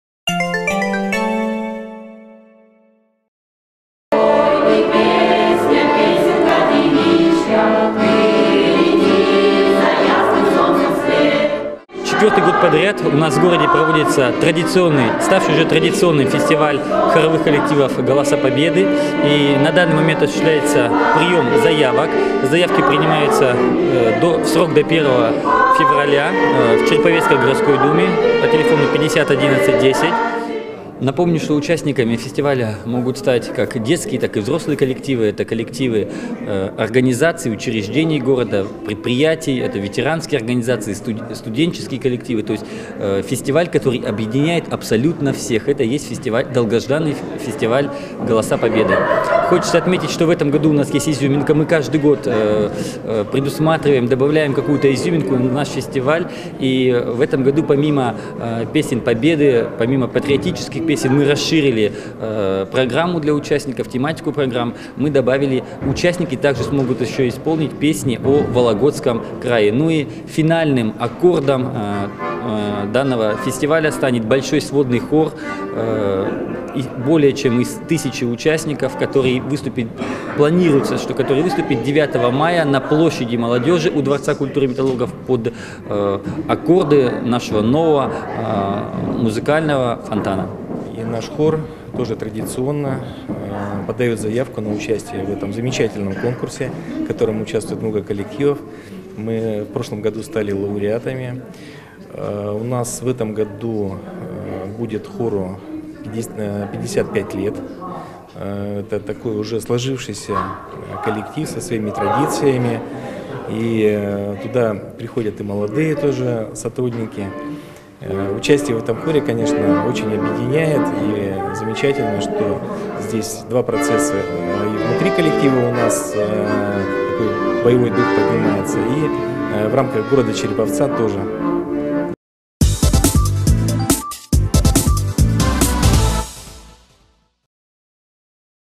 Запись репетиции хора МСЧ «Северсталь» для участия в фестивале «Голоса Победы»
В Череповце участники фестиваля хоров «Голоса Победы» споют песни о Вологодчине